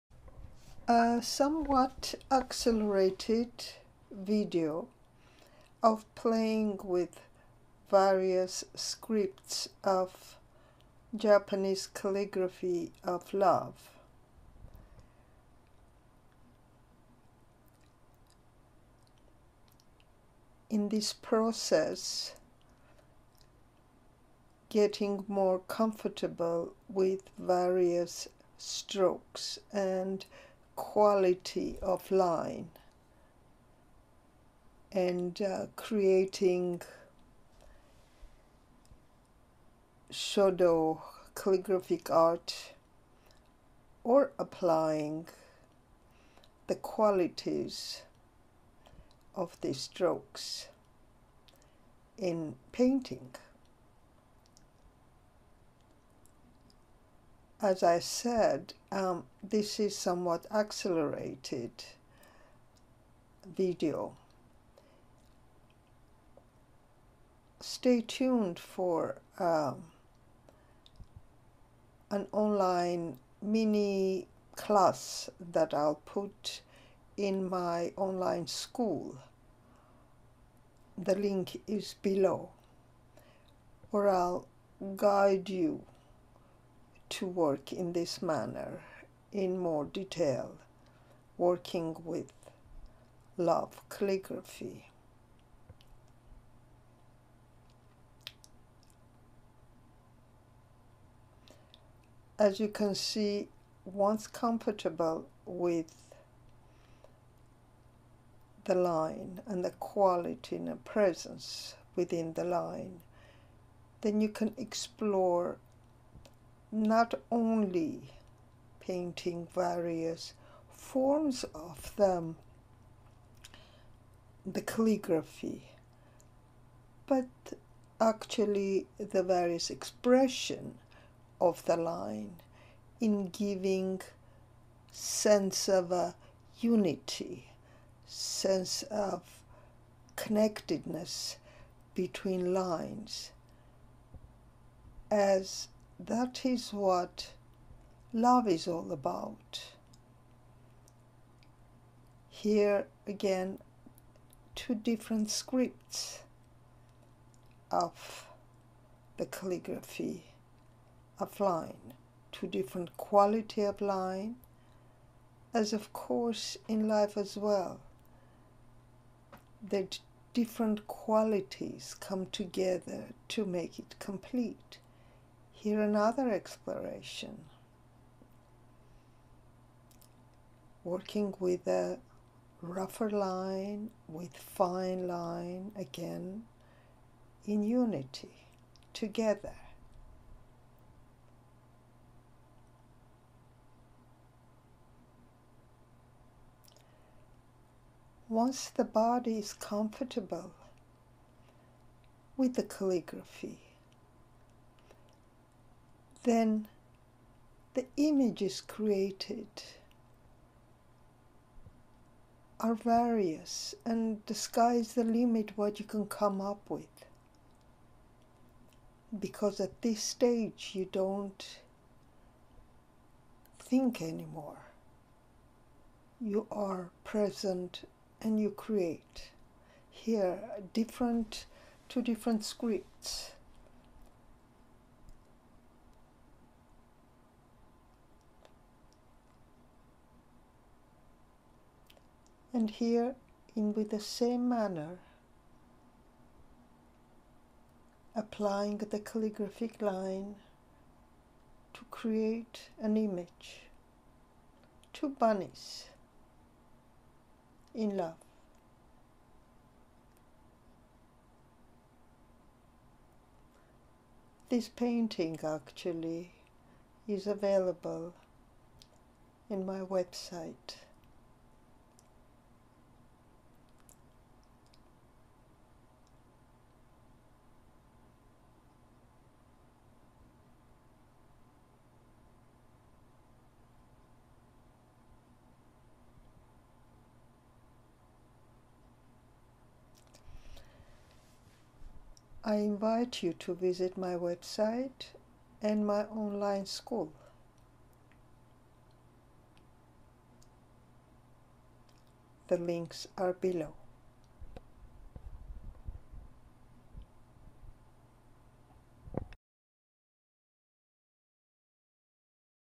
Japanese Calligraphy of "Love" in Shodo and Sumi-e
A demonstration of the method I teach and use to paint Sumi-e. It is the most effective yet authentic method for the westerner to learn the way and obtain the means of painting Sumi-e or in any other oriental style. With the practice of the calligraphic scripts and qualities of strokes, we create Shodo (Japanese calligraphic art), abstract or Sumi-e paintings.
If you would like to learn how to paint Sumi-e or any other painting in oriental style and spirit, I invite you to visit my online school.